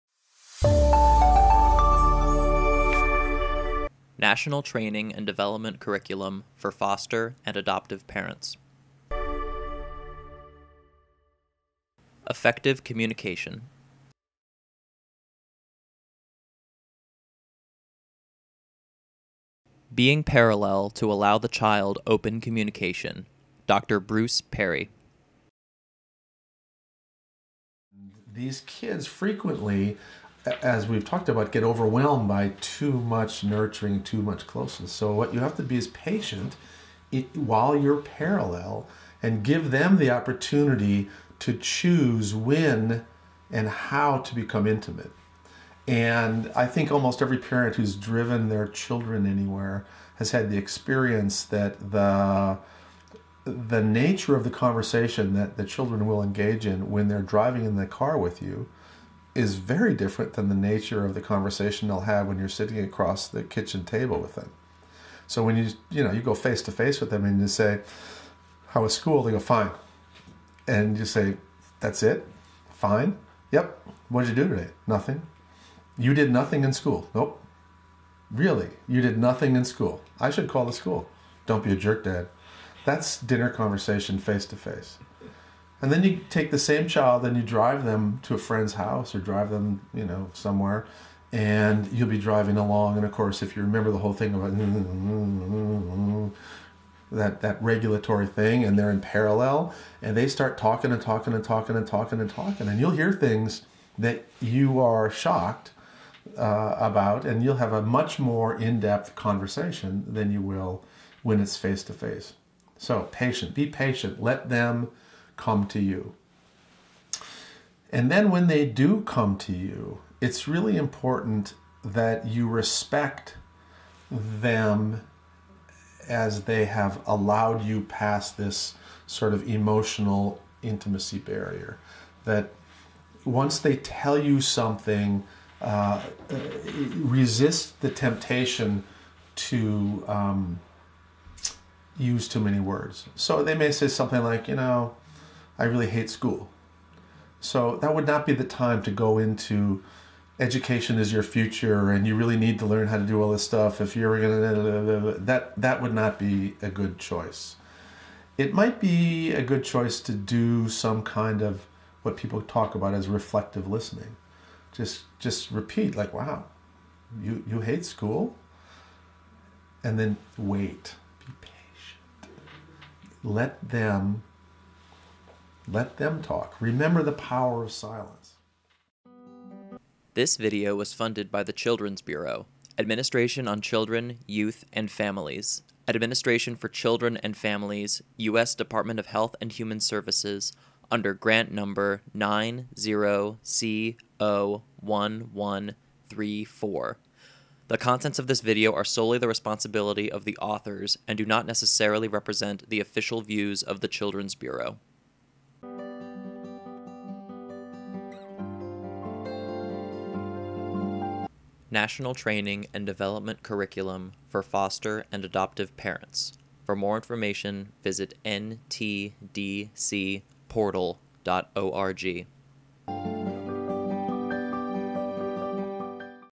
NTDC Classroom: Effective Communication - Being Parallel to Allow for Effective Communication (AD)
This video is part of the Effective Communication classroom-based training theme in the NTDC curriculum. In this short clip, Dr. Bruce Perry gives tips about the time, place and way to have meaningful conversations with children.

Dr. Perry is Senior Fellow and founder of the Child Trauma Academy in Houston, Texas and adjunct professor at the Department of Psychiatry and Behavioral Sciences at the Feinberg School of Medicine at Northwestern University in Chicago.